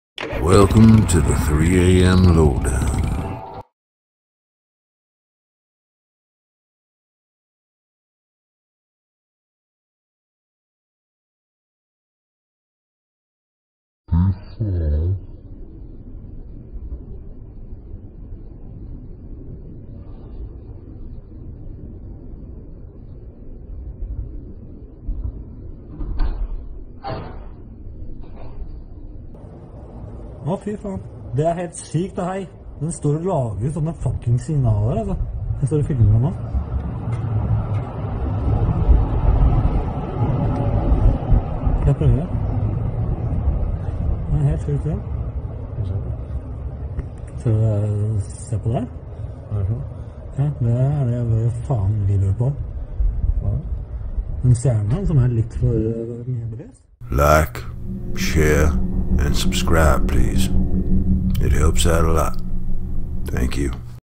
UFO sighting over Oslo, Norway 12/23/2020 - [12/26/2020]
3AM LOWDOWN DOES NOT CLAIM ANY RIGHTS OVER ANY OF THE GRAPHICS, IMAGES, VIDEOS, SONGS USED IN THIS VIDEO.

Source(s):
Caught over Oslo it looked like it was searching for something
@cityboymc 12/23/2020